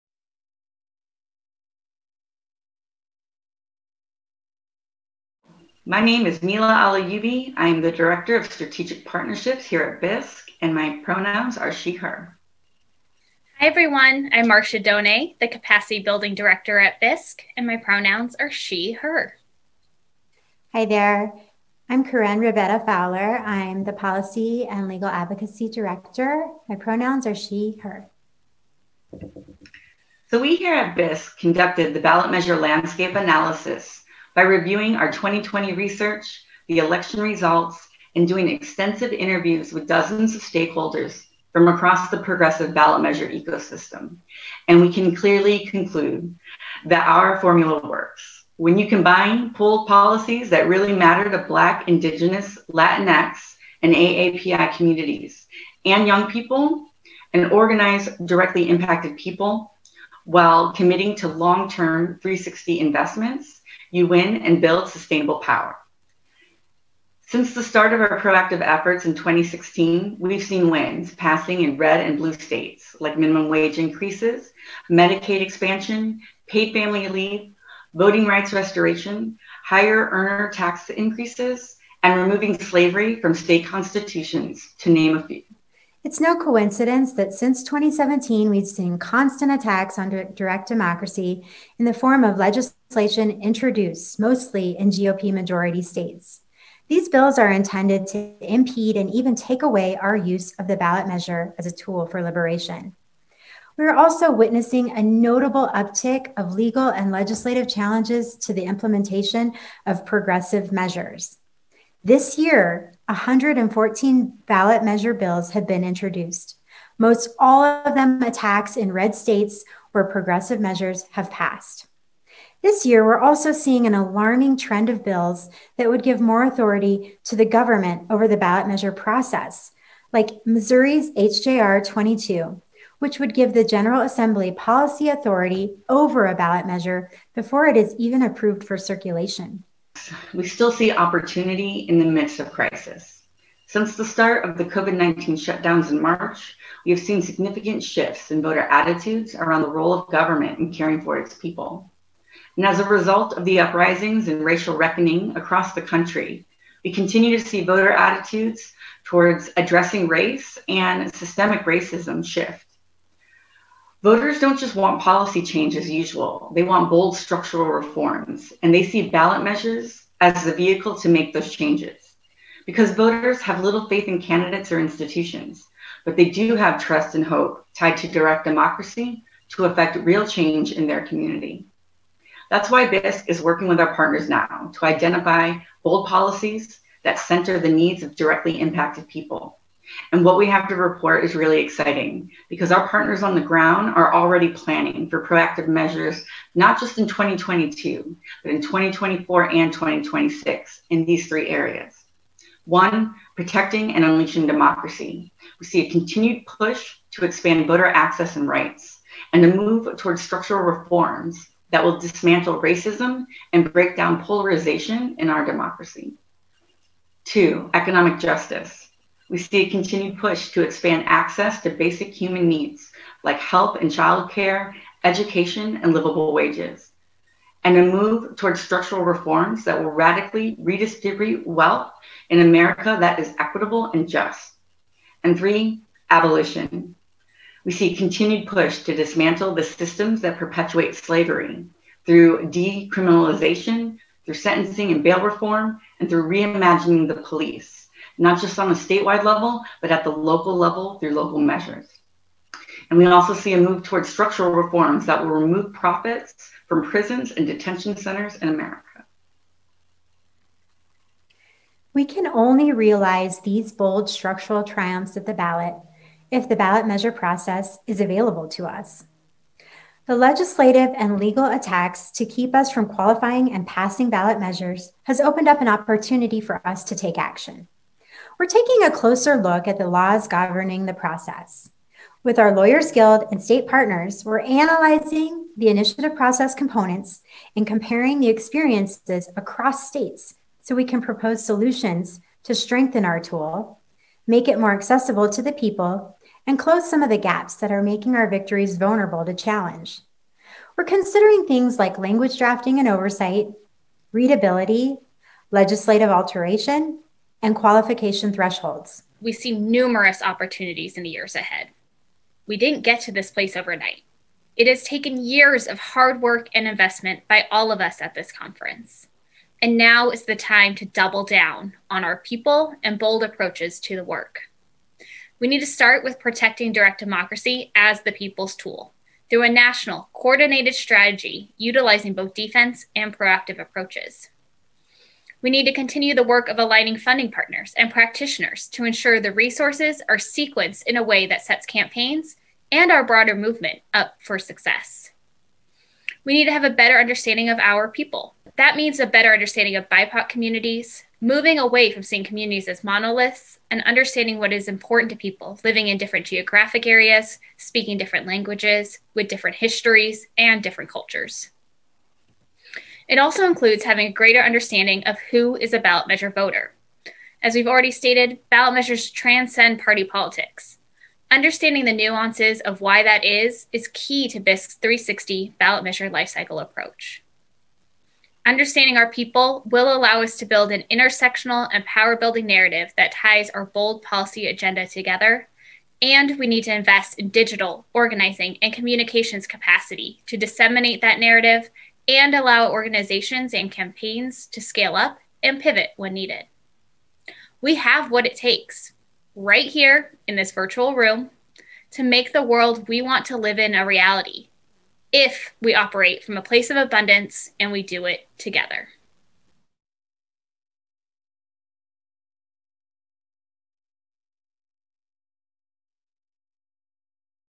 FORECASTING THE FUTURE: Ballot Measure Trends to Watch
Hear the latest from BISC staff on geographic and issue trends, opposition trends, and emerging innovations and methods to utilize this upcoming election cycle